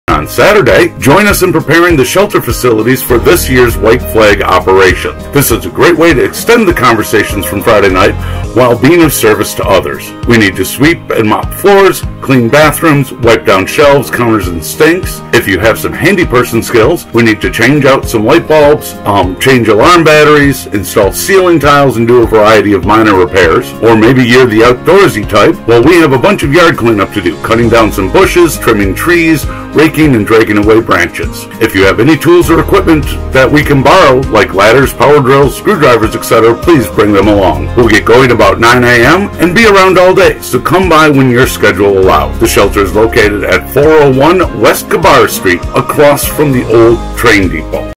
White Flag Shelter Prep on Saturday October 21, 2023
On Saturday, October 21st, join us in preparing the shelter facility for this year’s White Flag operation. This is a great way to extend the conversations from Friday while being of service to others.

We need to sweep and mop floors, clean bathrooms, wipe down shelves, counters, and sinks. If you have some handyperson skills, we need to change out some light bulbs, change alarm batteries, install ceiling tiles, and do a variety of minor repairs.

Or maybe you’re the outdoorsy type, well we have a bunch of yard clean up to do: cutting down some bushes, trimming trees, raking, and dragging away branches.

If you have any tools or equipment that we can borrow ... like ladders, power drills, screwdrivers, etc. ... please bring them along.

We’ll get going around 9am, and be around all day, so come by when your schedule allows.

The shelter is located at 401 West Cabarrus Street in Raleigh, across from the old train depot.